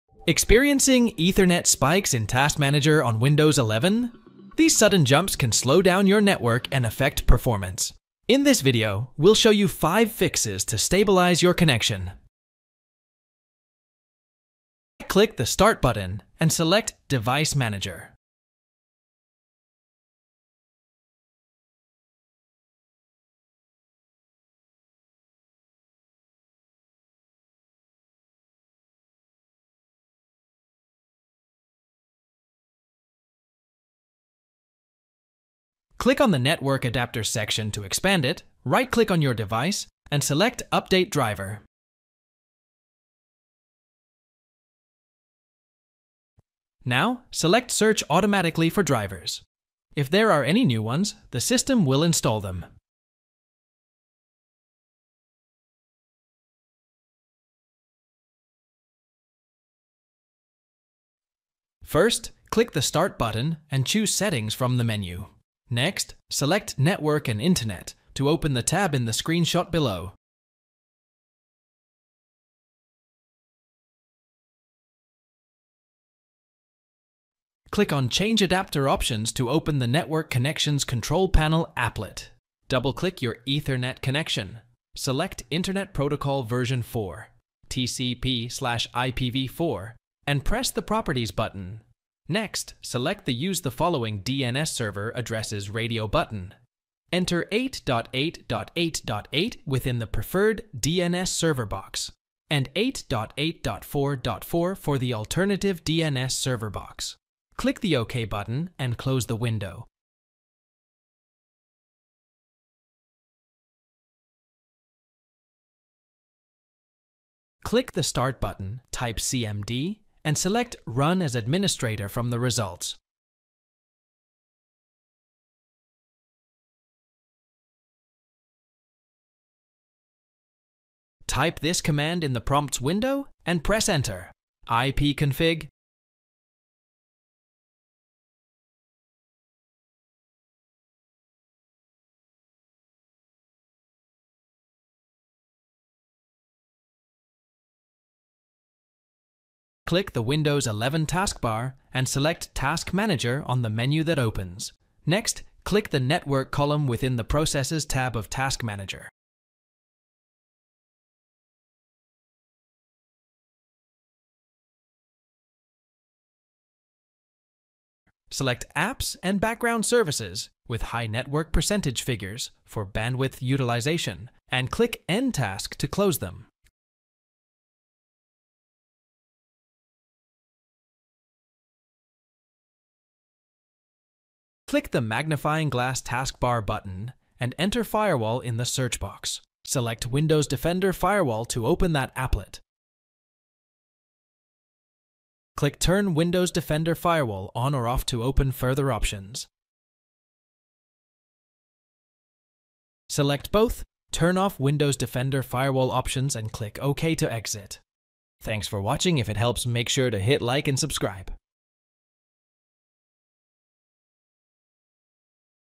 Ethernet Spiking In Task Manager Windows 11 - 5 Fix
Are you noticing unexpected Ethernet spikes in your Windows 11 Task Manager? These sudden increases in network activity can be frustrating and may hinder your online experience. In this video, we explore the common causes behind Ethernet spiking, such as background applications, network congestion, outdated drivers, malware, and system processes. By understanding these factors, you can take the necessary steps to identify and resolve the root causes of Ethernet spiking.

00:00 Intro
00:10 Update your network Drivers
00:55 Change to Google DNS
01:51 Select the WPA2-PSK AES Protocol
02:23 Close Bandwidth hogging background Programs
02:58 Reset the network settings using Command Prompt
03:29 Outro